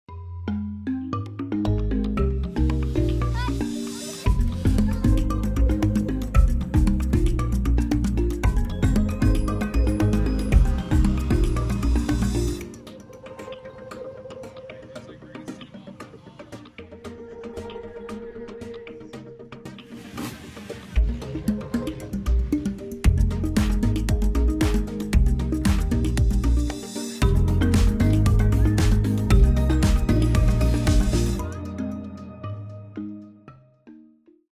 Motocross Family Time with Hudson the Hurricane! Like and subscribe y'all!
Wow! What a great time we had at the track! The bike let us down, but family did not! (Check out Hurricane's bike as he meets the checkered flag! Stall!). Never quit!